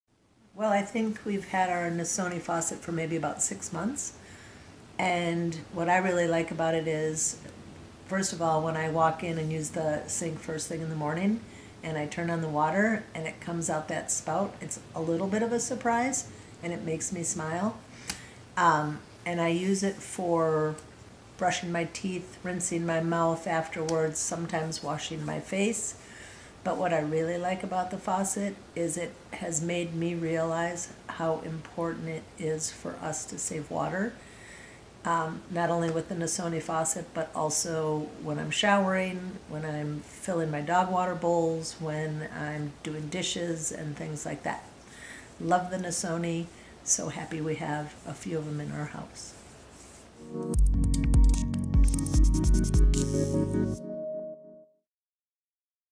Real People Real Stories, Barb / Minnesota
Nasoni customers talk about what they like best about Nasoni fountain faucets, and how they have improved their daily lives.